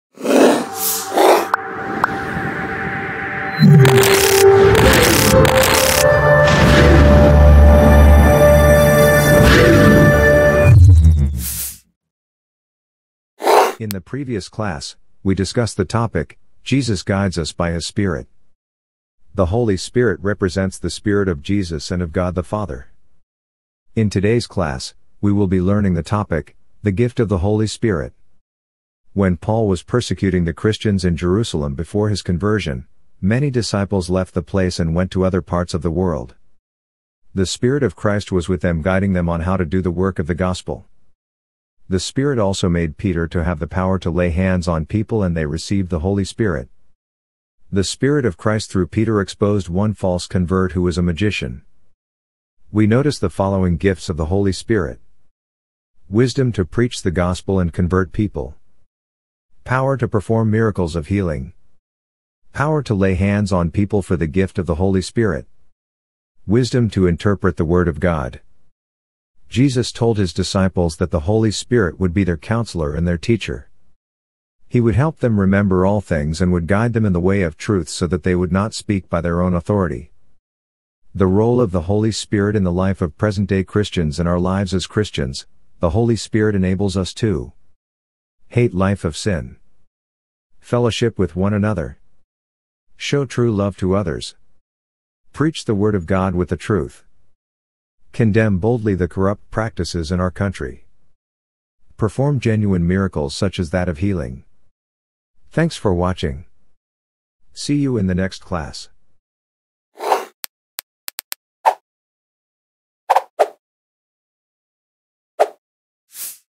The gift of the Holy Spirit
When Paul was persecuting the Christians in Jerusalem before his conversion, many disciples left the place and went to other parts of the world. The Spirit of Christ was with them guiding them on how to do the work of the Gospel.

An example was Philip who went to Samaria and preached the Gospel. The Spirit of Christ in him made him convert many people and to baptize them in the name of Jesus. The Spirit of Christ also gave him the power to heal the lame and blind people. He also cast demons from those that were possessed.